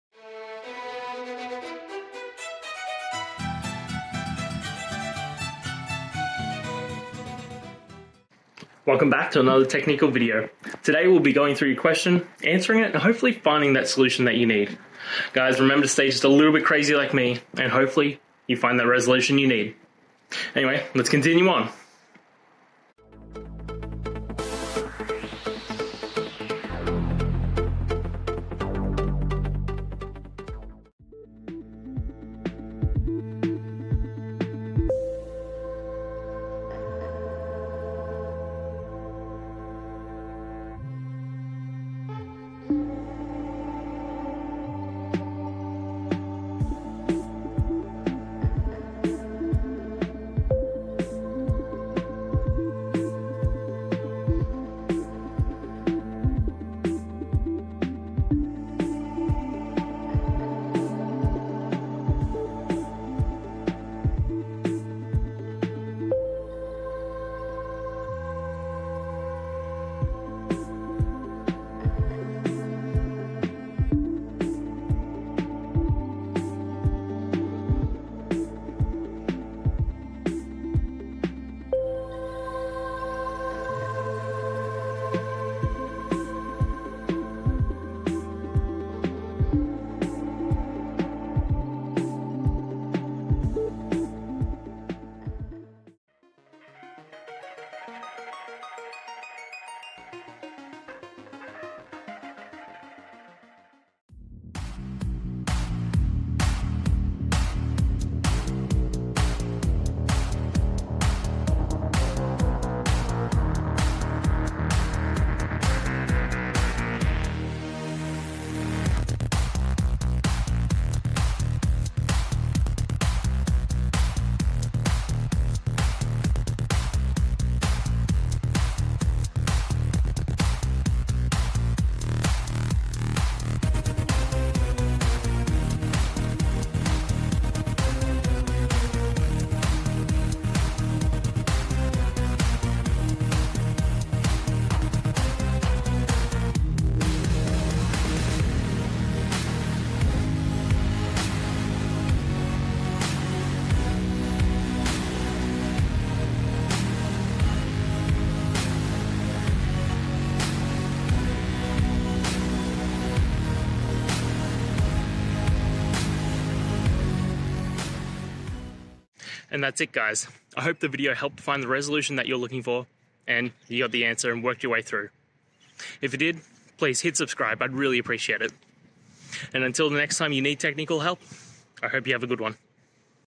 How to Set Status Code in AuthorizationPolicy: A Step-by-Step Guide
In this video, we’ll explore the essential topic of setting status codes within AuthorizationPolicy in your applications. Understanding how to properly configure these status codes is crucial for managing user access and ensuring a secure environment. Join us as we walk through a step-by-step guide that will empower you to implement these settings effectively, enhancing both your application's functionality and user experience.

Today's Topic: How to Set Status Code in AuthorizationPolicy: A Step-by-Step Guide

Related to: setstatuscode, authorizationpolicy, step-by-stepguide,